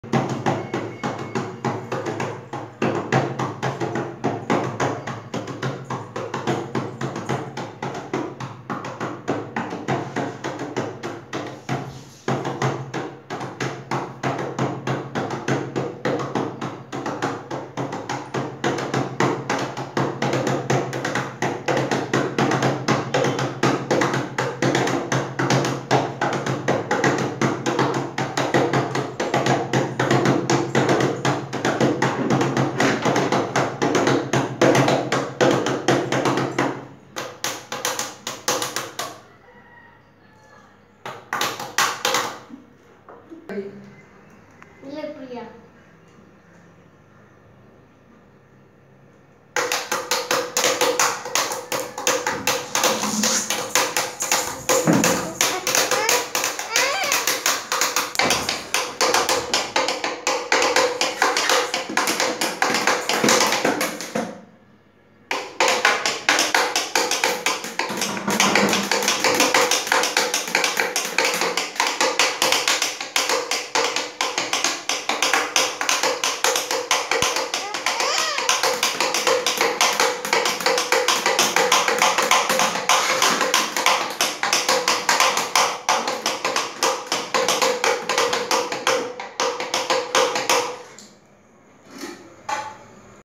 My son playing music with stool,comb,pencil...
Stool,comb,pencil,how is it